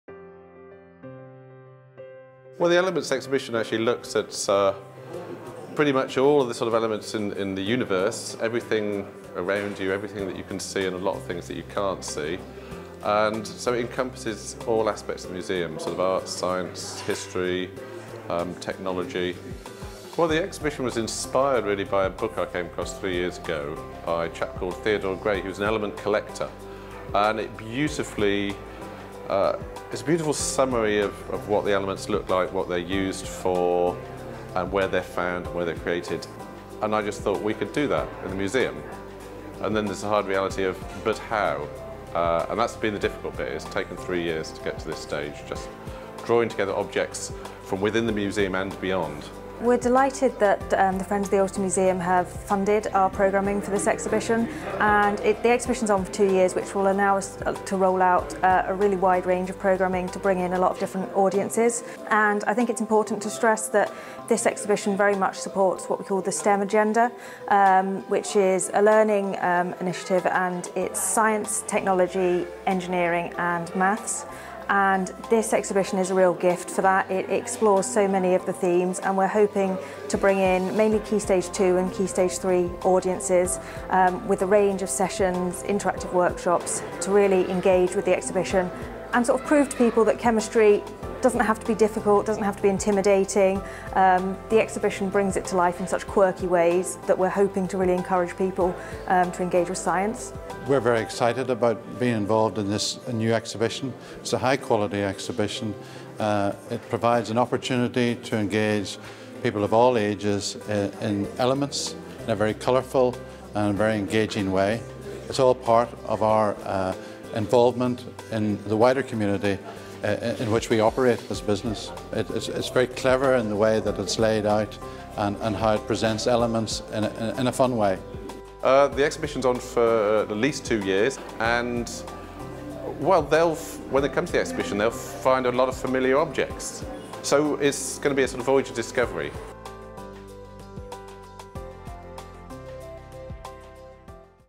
Elements Exhibition Ulster Museum
Introduction to the new Elements exhibition in the Ulster Museum, covering the periodic table in a fun and engaging way. Exhibition Curator Mike Simms explains where the idea for the exhibition came from and gives a brief description of the items on display.